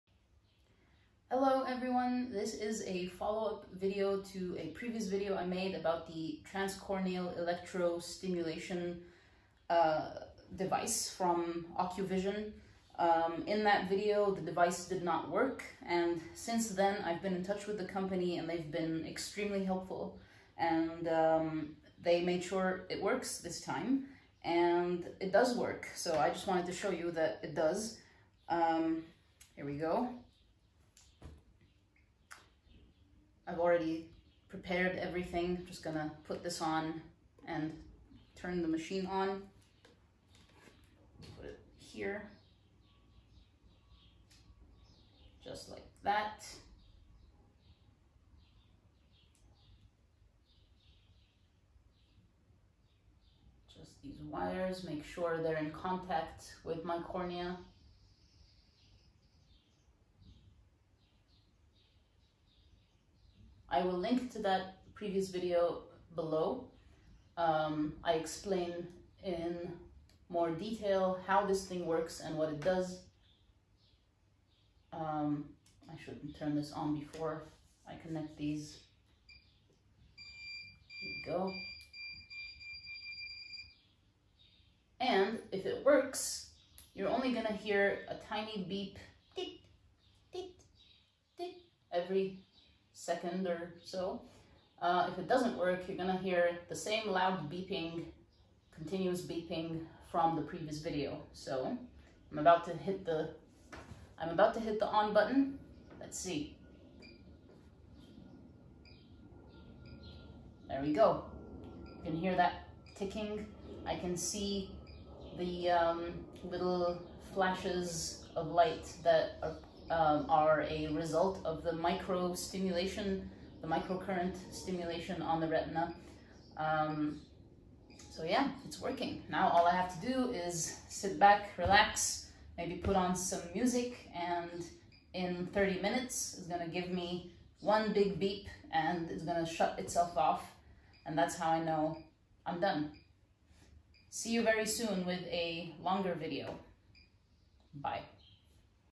Follow up on the trans-corneal electrostimulation device by Okuvision￼
Many thanks to the people at Okuvision in Germany for helping me get my device to work again￼

Microcurrent stimulation of the retina is one of the modern treatments available to patients with retinal conditions to help them preserve their remaining vision and allow the retina￼s to function at their best￼￼.